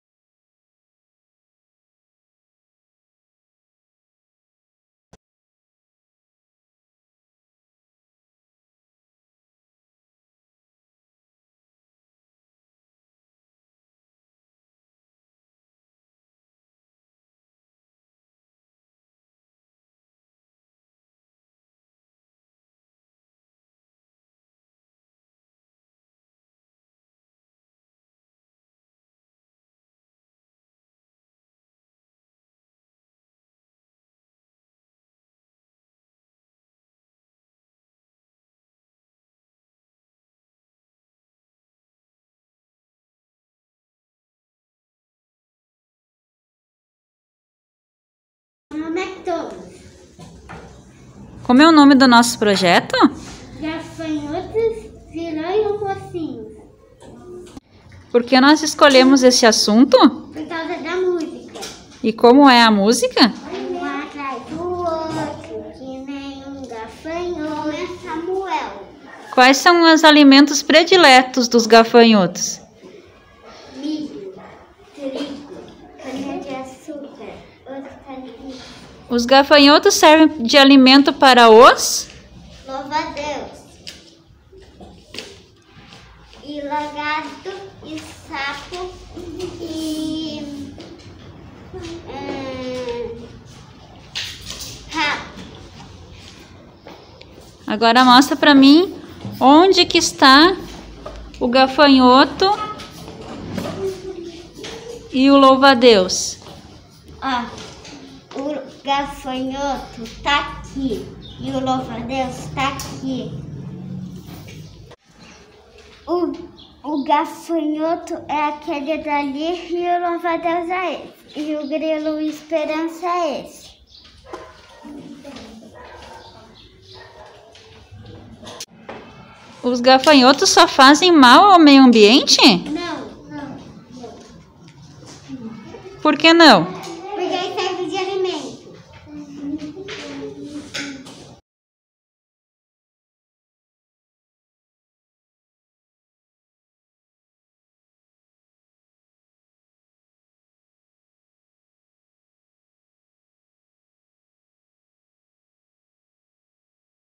15A - GAFANHOTOS, SÃO VILÕES OU MOCINHOS? - EMEF Genuíno Sampaio - Pré 1 e 2
O projeto busca conhecer a vida desses insetos famintos, que em junho de 2020, se tornaram grande preocupação para os agricultores do país. Os gafanhotos são mal vistos por serem destruidores, mas o desequilíbrio do meio ambiente é a principal causa da redução dos predadores. Eles são importantes dentro da cadeia alimentar de outros insetos e animais.